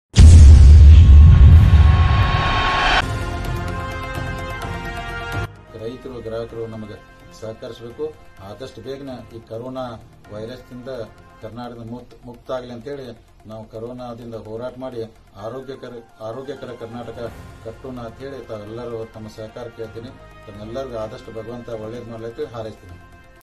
fast 9 news kannada
ಕೆ ಎಮ್ ಎಫ್ ರಾಜ್ಯಾಧ್ಯಕ್ಷ ಚಾಲಚಂದ್ರ ಜಾರಕಿಹೊಳಿ ಕೊರೊನಾ ಜಾಗೃತಿ